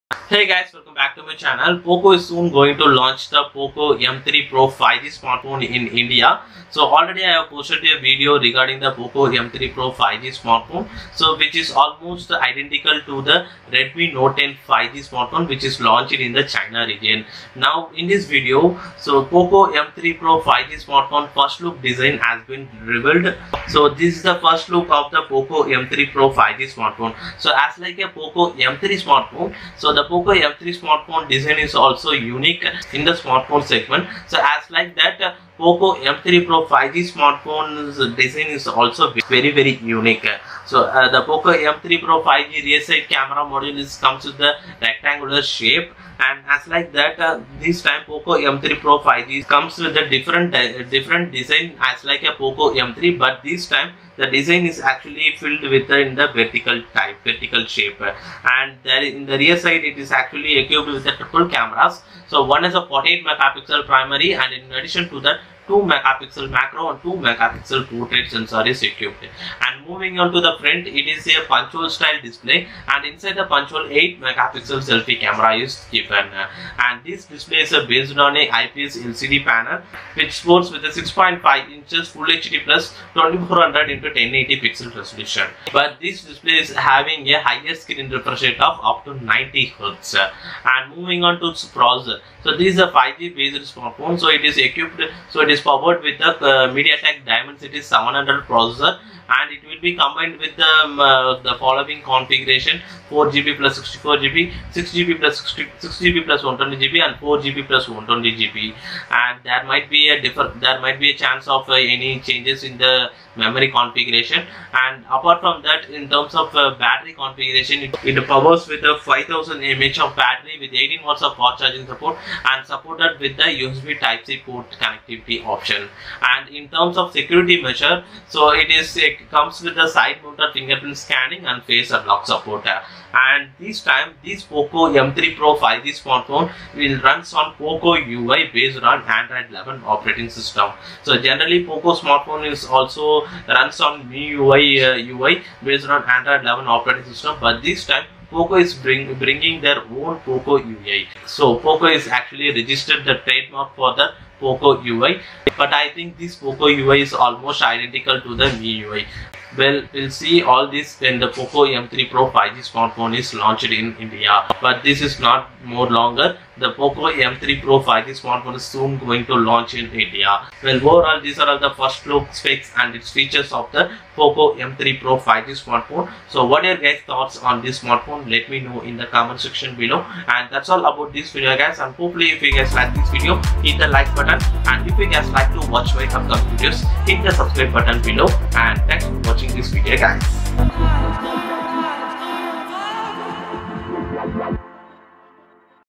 POCO M3 Pro 5G First Look & Specifications 🔥 POCO UI 💥 India Launch Soon
Hey Guys, Welcome back to TechNtech Tamil,

In this video, let's find Poco M3 Pro 5G Smartphone first look & full specs got revealed in this video. POCO M3 Pro 5G will soon launch in India.